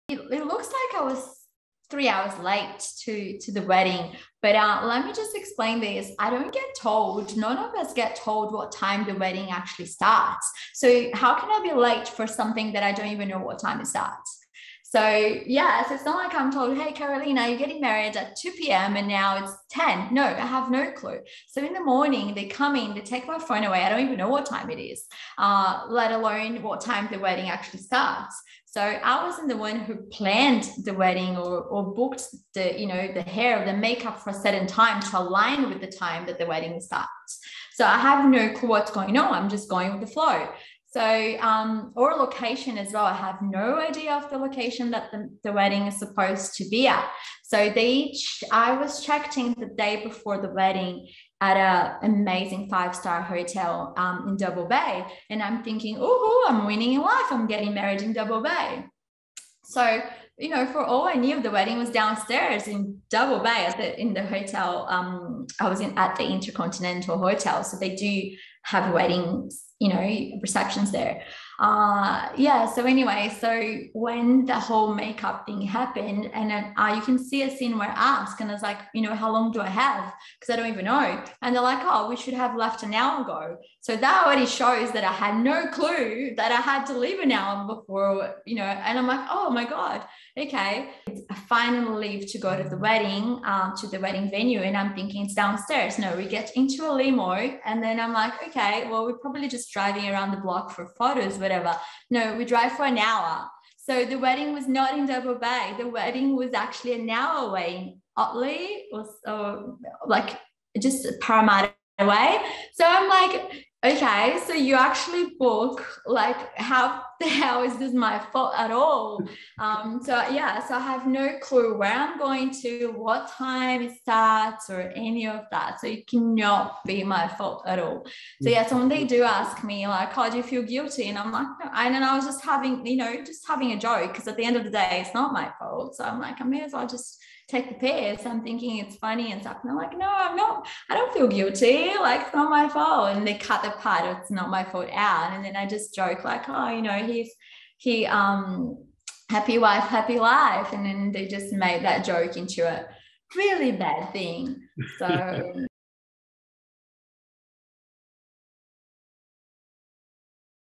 MAFS' Carolina explains why she was late to her wedding | Yahoo Australia
Married At First Sight Australia's Carolina reveals why she was three hours late to her wedding. Source: Yahoo Lifestyle Australia